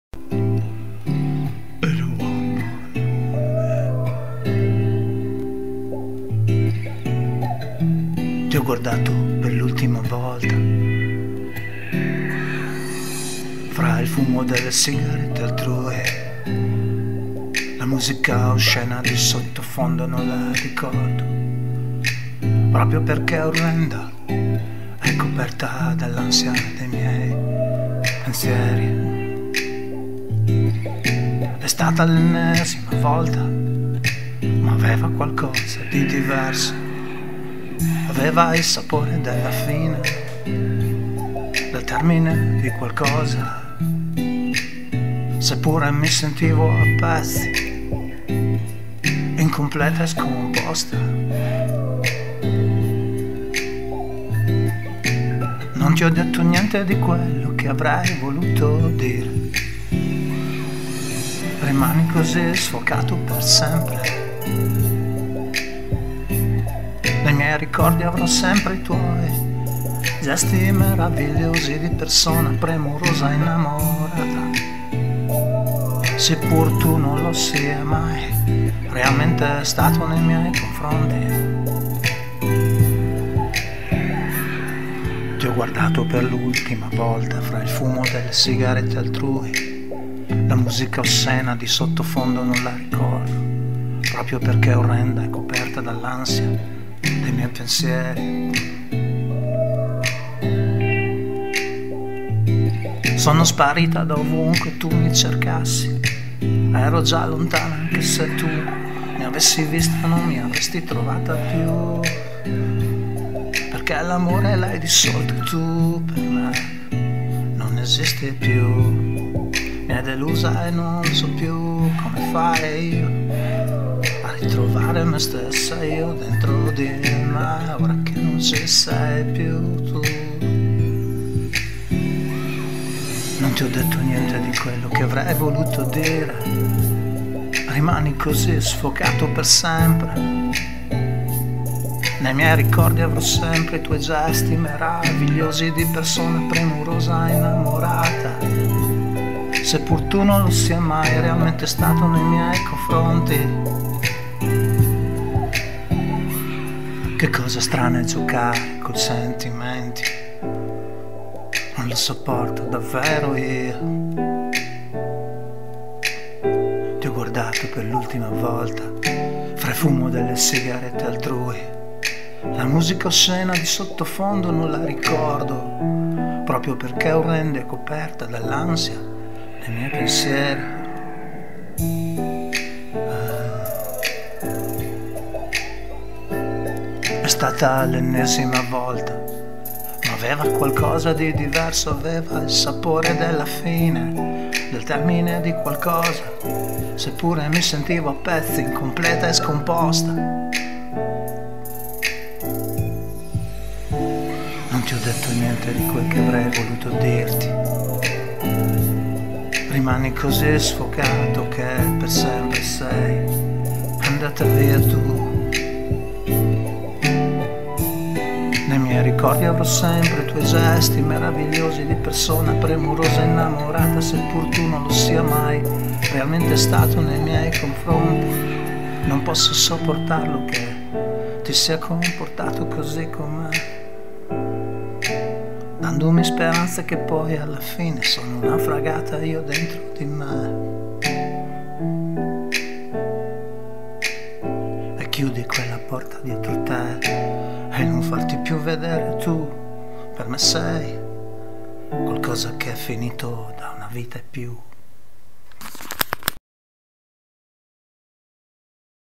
Ti guardato per l'ultima volta - Roberto Piana
Ti ho guardato per l’ultima volta, fra il fumo delle sigarette altrui. La musica oscena di sottofondo non la ricordo, proprio perché orrenda e coperta dall’ansia dei miei pensieri.

È stata l’ennesima ultima volta, ma aveva qualcosa di diverso, aveva il sapore della fine, del termine di qualcosa, seppure mi sentivo a pezzi, incompleta e scomposta.

Non ti ho detto niente di quello che avrei voluto dire. Rimani così sfuocato per sempre.

Nei miei ricordi avrò sempre i tuoi gesti meravigliosi di persona premurosa e innamorata, seppur tu non lo sia mai realmente stato nei miei confronti.